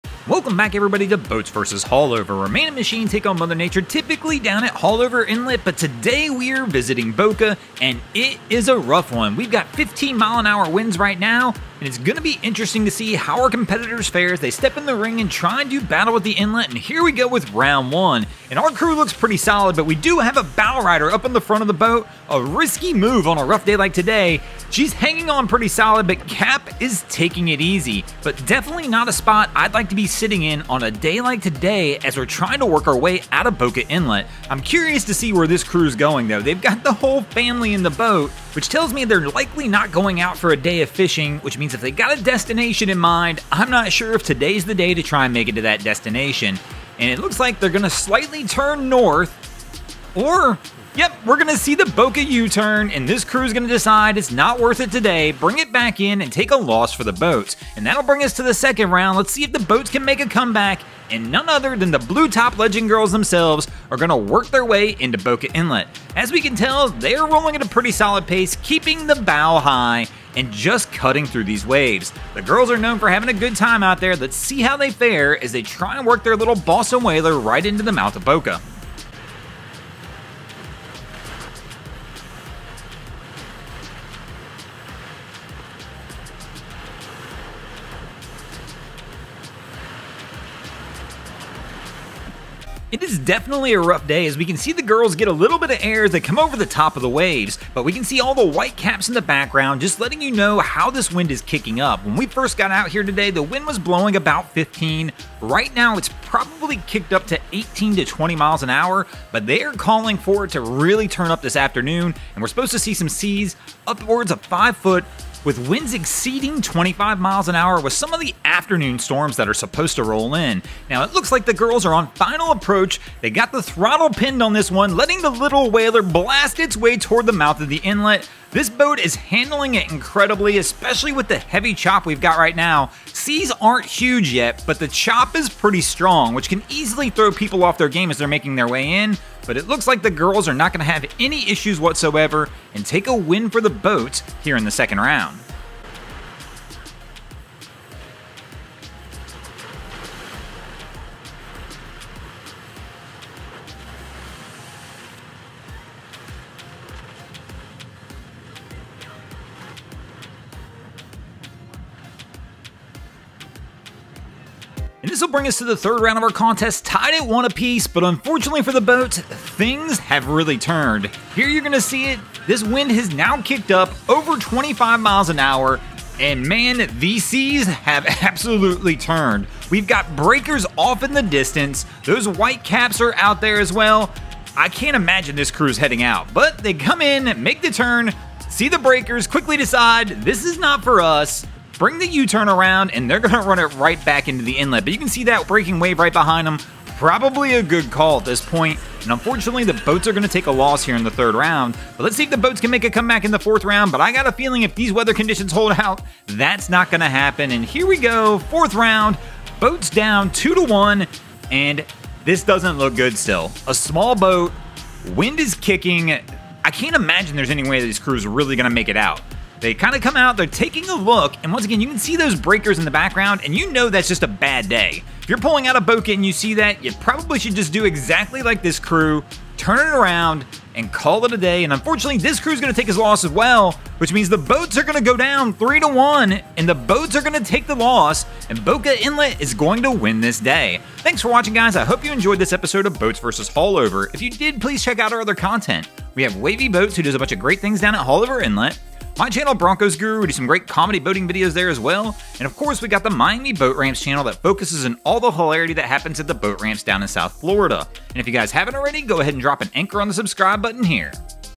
SMALL BOAT IN PERILOUS WATERS AT BOCA INLET ! | Boats vs Haulover Inlet
Welcome back to another episode where man and machine attempt to do battle with Haulover Inlet!

This episode we are back home at Haulover Inlet. Who will take the win this week between Haulover Inlet and the Boats?

We upload videos twice a week on Saturdays and Wednesdays.